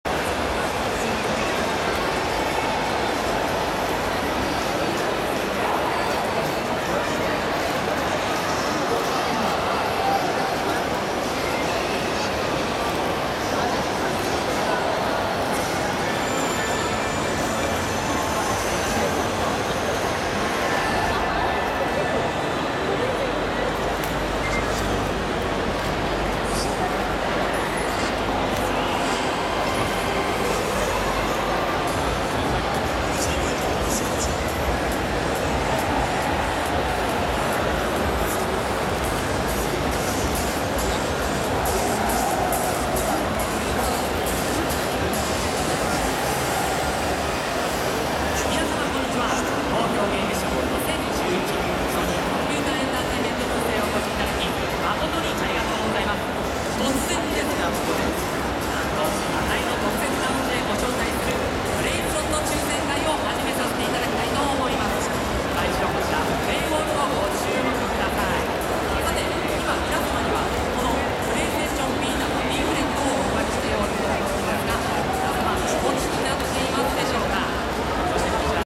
Popular cosplayer at TGS 2011
This cosplayer was attracting a very large group of photographers.